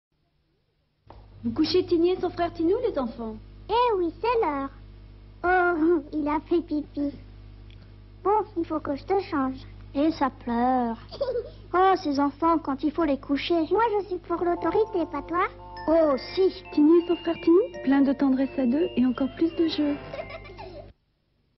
Tinnie y Tinou (1989)
Anuncio de los muñecos Tinnie y Tinou, fabricados por Tonka. Emitido en Francia en 1989.

Extraído de: ina.fr